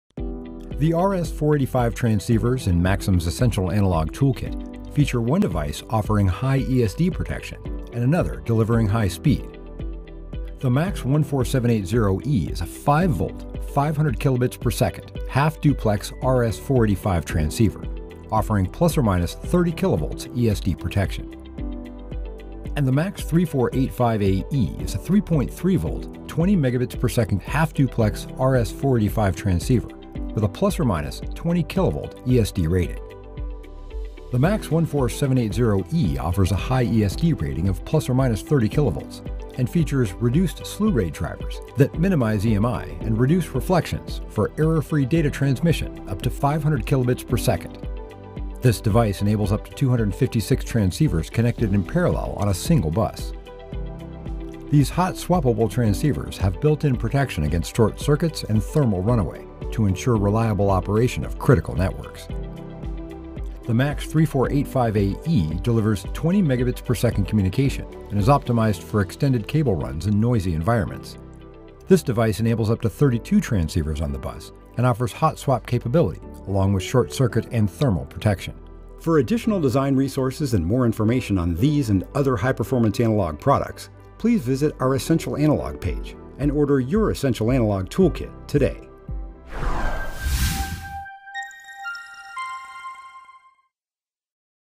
Essential Analog Toolkit: RS-485 Transceivers
Learn about the features, benefits, and applications of the RS-485 transceivers included in the Essential Analog Toolkit. The nanopower MAX14780E is a 5V half-duplex transceiver that supports a 500kbps data rate with ±30kV of ESD protection. The MAX3485AE is a 3.3V transceiver with a 20Mbps data rate and a ±20kV ESD rating.